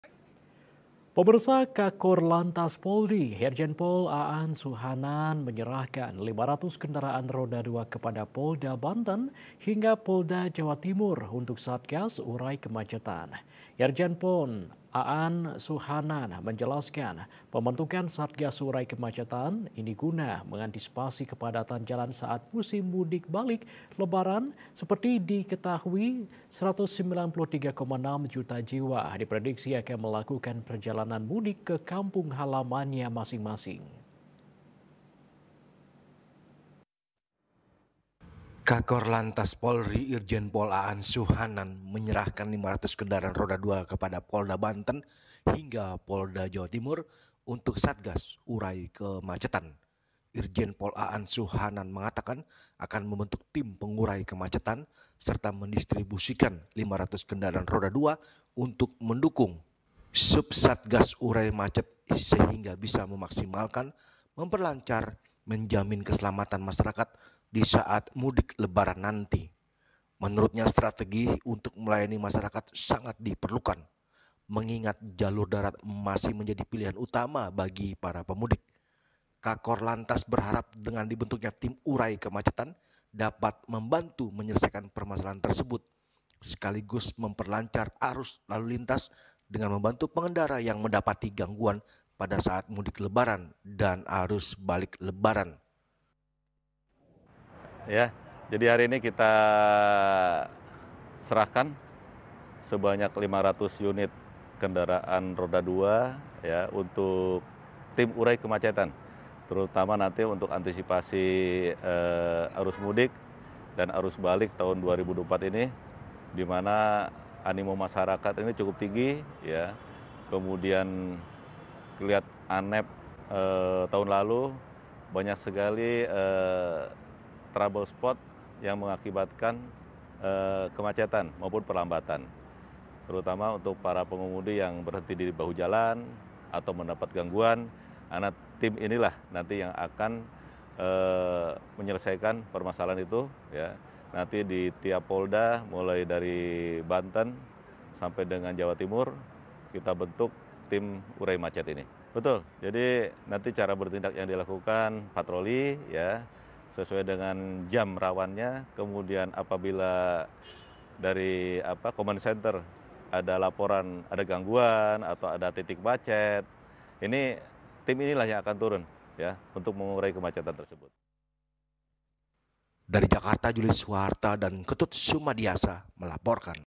SUKSESKAN MUDIK LEBARAN KAKORLANTAS SERAHKAN 500 KENDARAAN RODA DUA UNTUK SATGAS URAI KEMACETAN
KAKORLANTAS POLRI, IRJEN POL AAN SUHANAN ,MENYERAHKAN 500 KENDARAAN RODA DUA KEPADA, POLDA BANTEN ,HINGGA POLDA JAWA TIMUR, UNTUK SATGAS, URAI KEMACETAN.IRJEN PON AAN SUHANAN MENJELASKAN ,PEMBENTUKAN SATGAS URAI KEMACETAN, INI GUNA MENGANTISIPASI KEPADATAN JALAN SAAT, MUSIM MUDIK BALIK LEBARAN.SEPERTI DI KETAHUI 193,6 JUTA JIWA, DI PREDIKSI AKAN MELAKUKAN PERJALANAN, MUDIK KE KAMPUNG HALAMANNYA MASING- MASING.